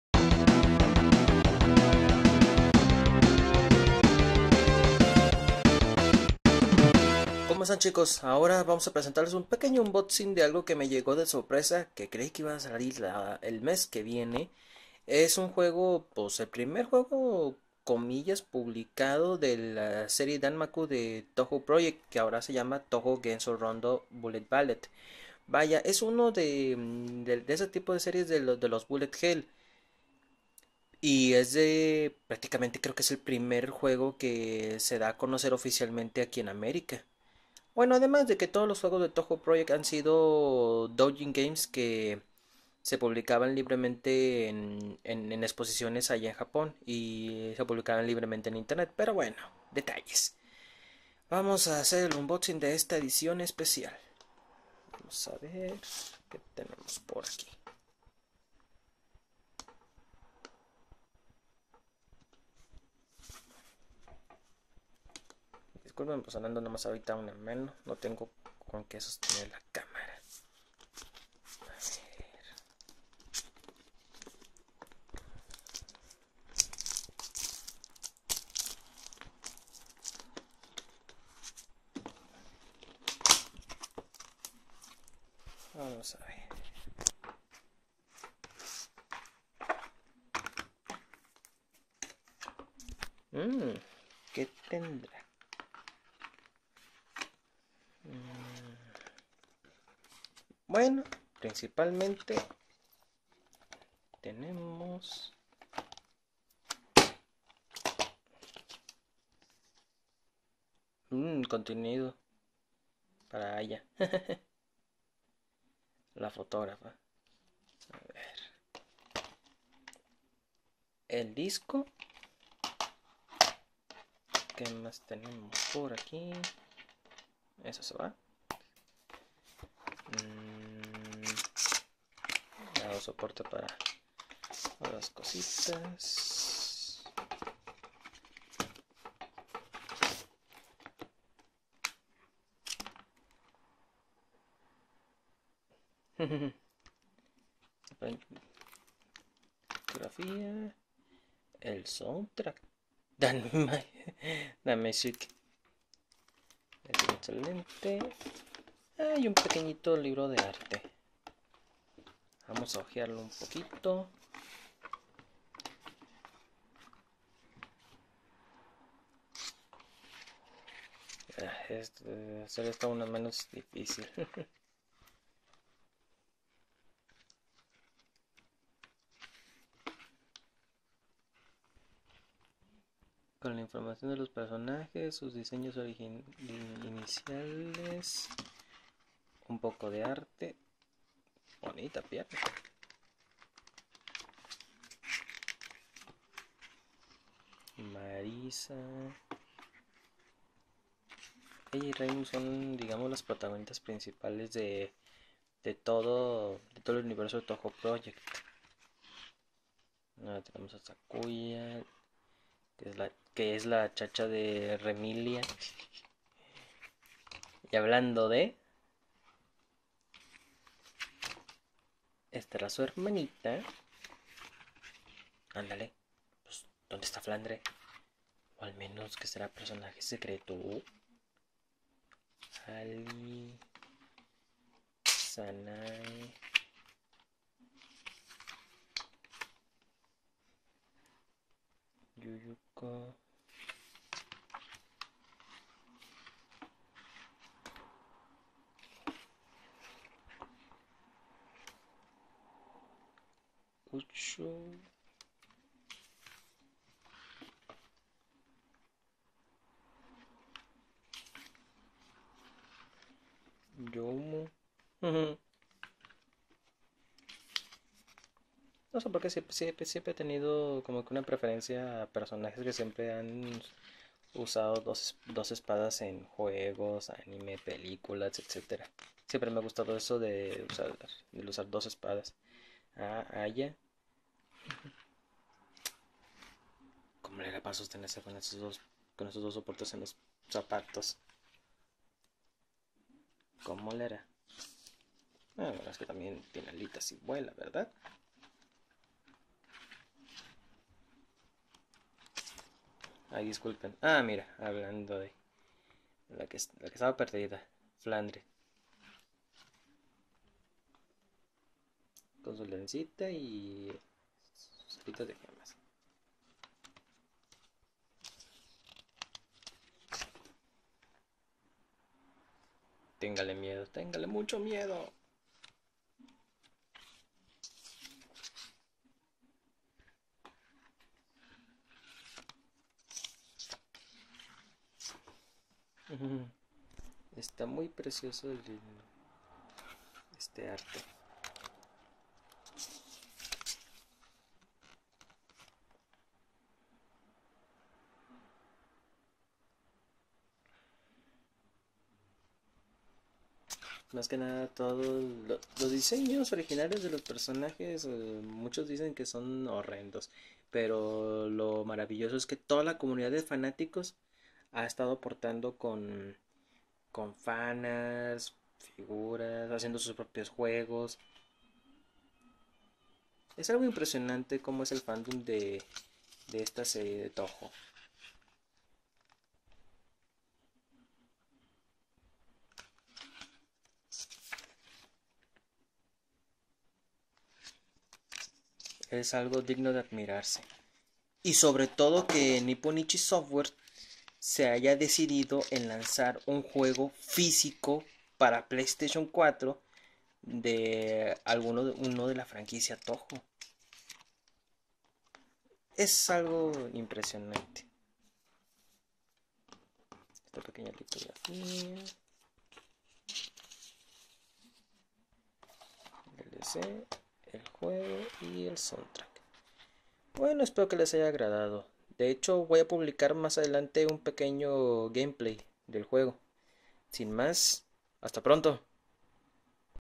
Unboxing Touhou Genso Rondo - Bullet Ballet
Les comparto el unboxing de Touhou Genso Rondo - Bullet Ballet

Uno de los primeros juegos de Touhou Project publicado en america gracias a NIS America